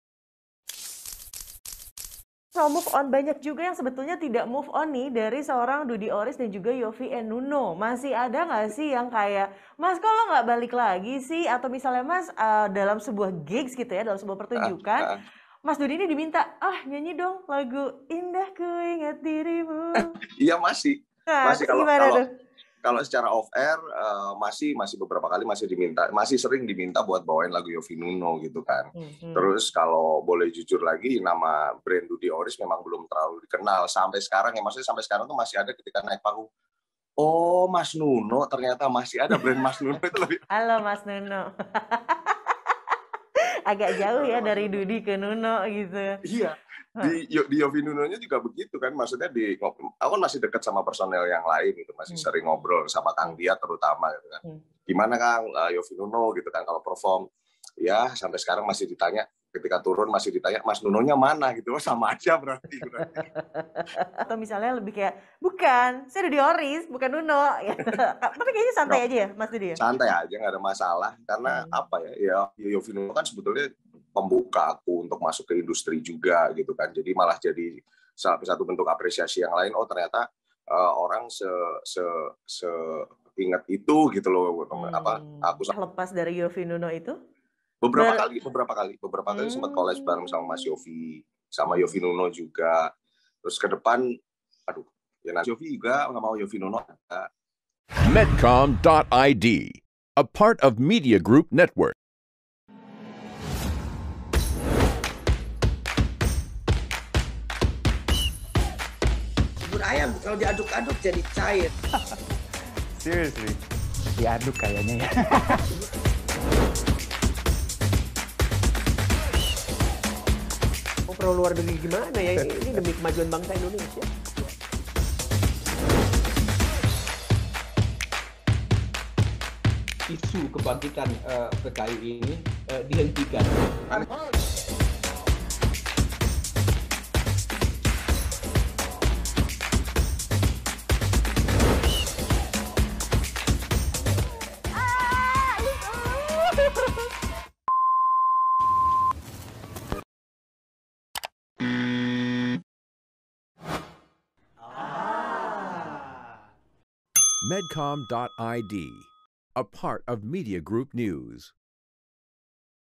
Cerita Dudy Oris Masih Sering Diminta Fans Nyanyi Lagu Yovie & Nuno
Dunia musik Tanah Air era 2000-an sarat dengan kehadiran para musikus yang lagu-lagunya dikenang hingga hari ini. Salah satunya adalah Yovie & Nuno yang formasi vokalnya sempat diisi Dudy Oris. Sejak hengkang pada 2012, Dudy Oris memilih untuk bekiprah sebagai penyanyi solo. Dalam perjalanan manggungnya, Dudy sering diminta fans untuk menyanyikan lagu Yovie & Nuno.

(ARV)